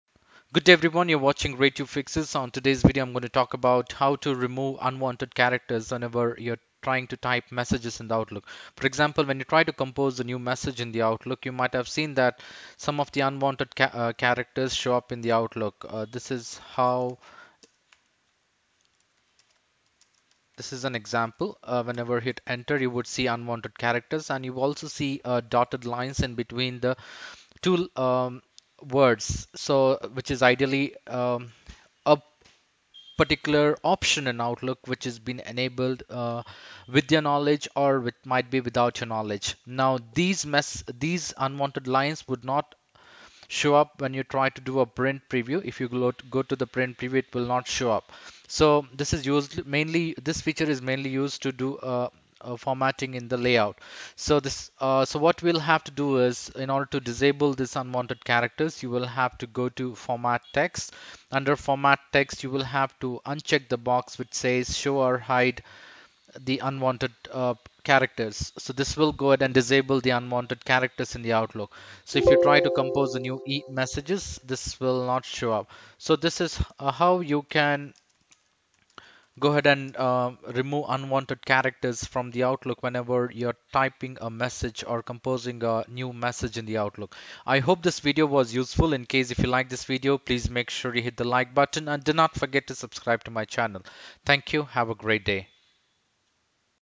FIX!!! HOW TO REMOVE UNWANTED CHARACTER SYMBOLS OR PARAGRAPH SYMBOL IN OUTLOOK
Turn formatting marks on or off

Applies To :
Outlook for Office 365 Outlook 2019 Outlook 2016 Outlook 2013 Outlook 2010 Outlook 2007

When composing an email message, you might see some symbols within your text. These are actually formatting marks, such as dots (for spaces) or arrows (for tab characters) in Outlook. Formatting marks assist with text layout. They do not appear on a printed message.

When you turn formatting marks on or off, your choice remains in effect for all subsequent messages you compose, reply to, or forward.

Quickly toggle formatting marks
To turn formatting marks on or off, do the following:

In the message window, on the Format Text tab, in the Paragraph group, click the button that looks like a paragraph mark. (When you point your mouse at the button, the tooltip says Show/Hide ¶).

Keyboard shortcut     CTRL+SHIFT+*. (You must use the asterisk on the 8 key.)